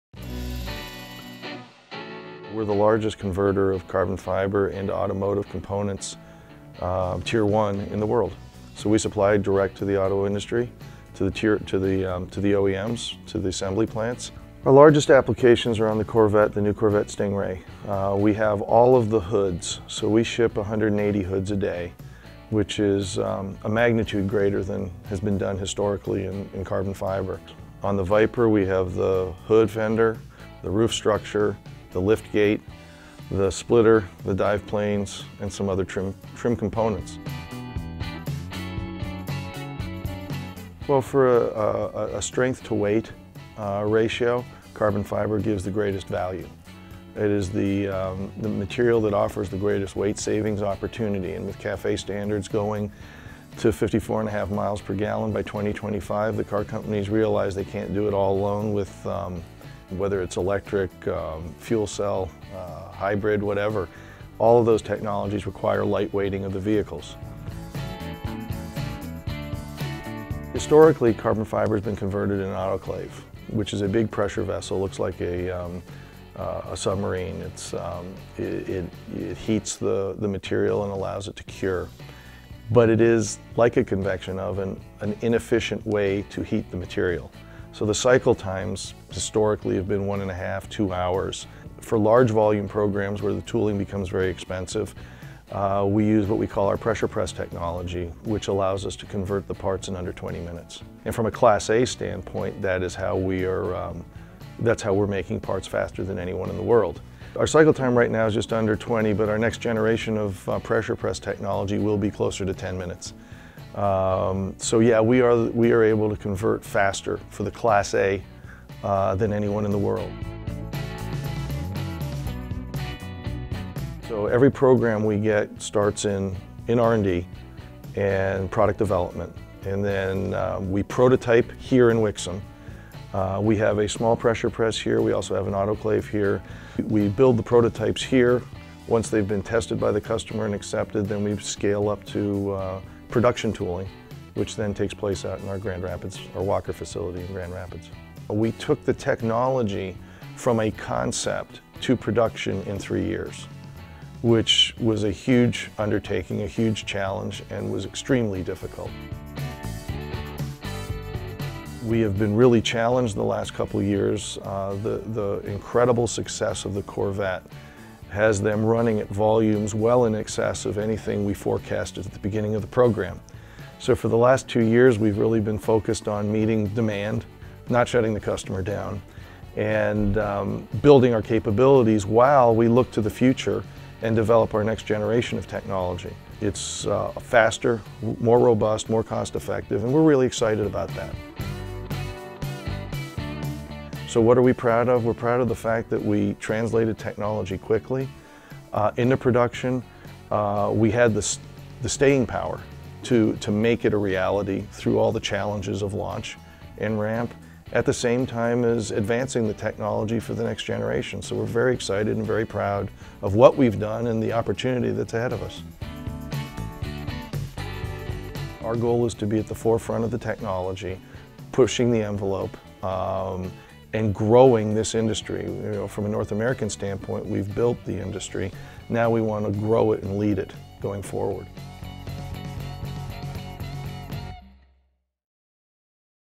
Plasan Carbon Composites Overview Video
Dedicated to automotive carbon fiber, Plasan Carbon Composites is the leading Tier 1 supplier of carbon fiber parts and assemblies in the United States.

Watch this video to learn more about what we’ve done, our process, and what we’re doing to improve carbon fiber mass reduction solutions.